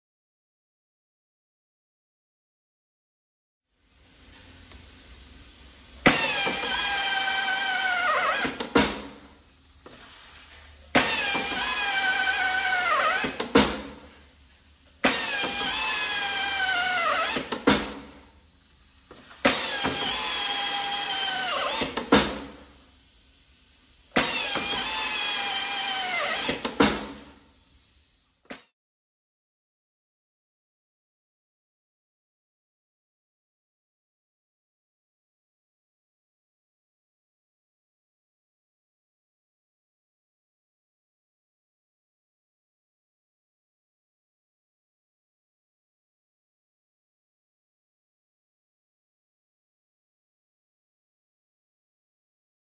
Wkrętarka jednogłowicowa POWASERT ADS-SE1
Film przedstawiający proces pracy maszyny POWASERT ADS-SE1